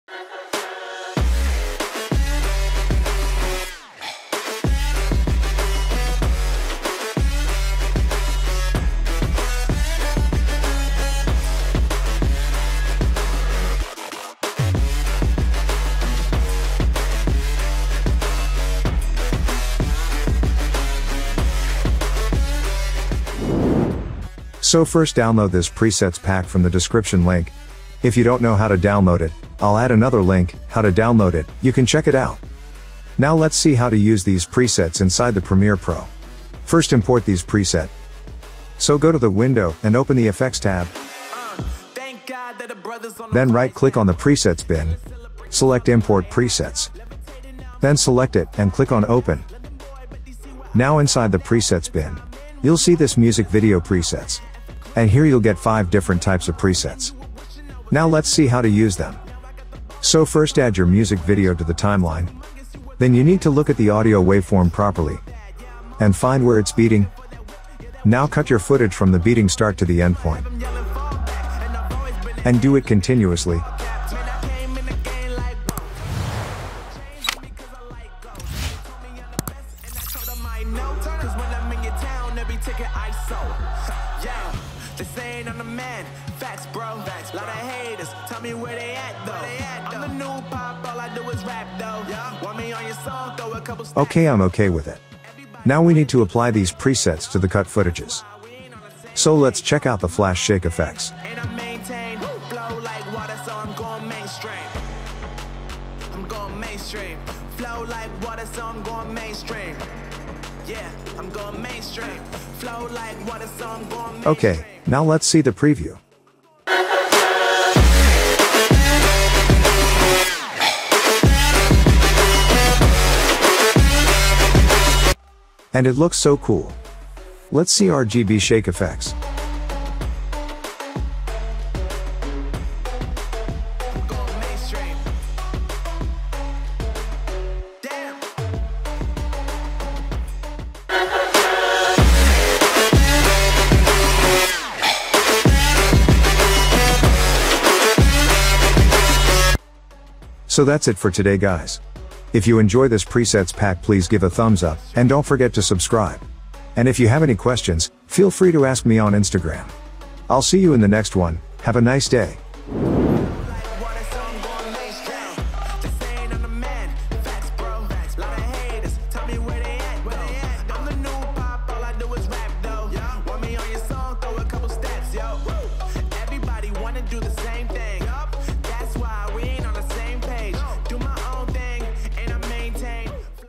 Music Video Effects - Premiere Pro Presets Pack (Tutorial)
➤ Today I'll give you the ultimate music video effect presets for the premiere pro. Easily you can create awesome music beat effects with this pack.

In this music video effects pack, You'll get 5 different types of presets:
➢ Flash Effects (10)
➢ RGB Shake Effects (10)
➢ Beat Shake Effects (10)
➢ Zoom Shake Effects (15)
➢ Strobe Light Effects (2)